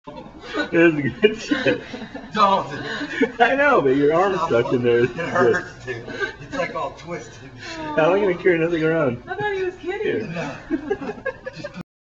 My neighbor - arm stuck in a fountain
Jason is a great guy. He was trying to clear a clog inside this fountain and got his arm stuck. It makes me laugh for many different reasons.